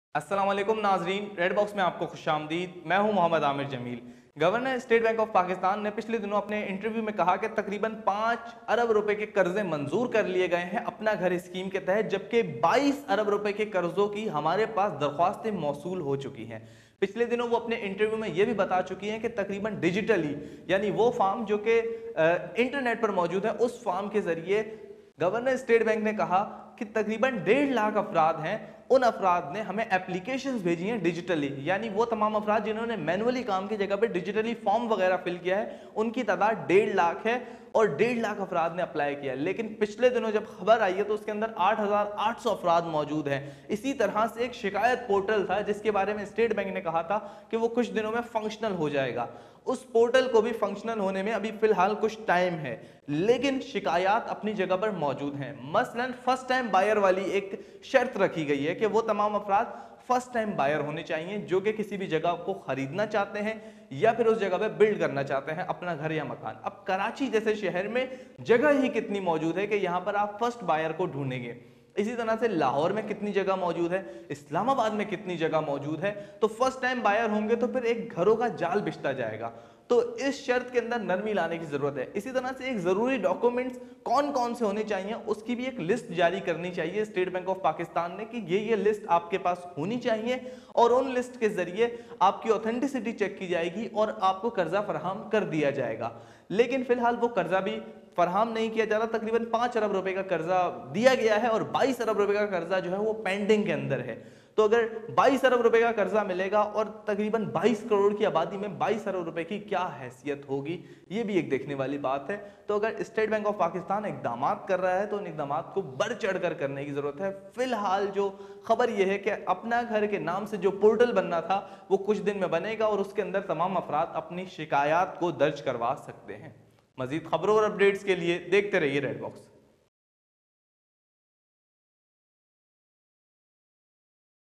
NEWS UPDATE: NAYA PAKISTAN HOUSING SCHEME | BANK LOAN | HOUSE | PM IMRAN KHAN | REGISTRATION FORM
A detailed video on Naya Pakistan Housing scheme complaint portal launched by PM IMRAN KHAN.